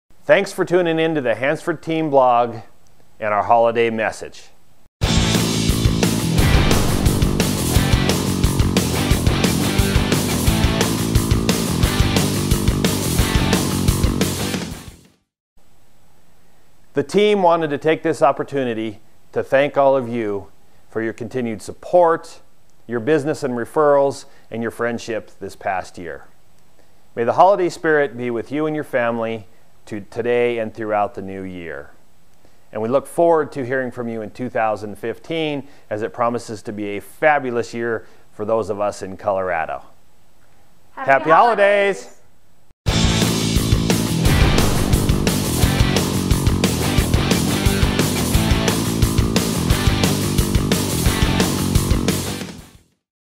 Denver Real Estate Agent: Season's greetings from Troy
- The holiday season is finally here! We had a great 2014 thanks to all of our great clients, partners, and referrals. We are looking forward to 2015 being even better, and it will be with your continued support! To see our team's special holiday message, watch this short video!

Troy Hansford
RE/MAX Unlimited
3300 S. Parker Rd. #100
Denver, CO 80014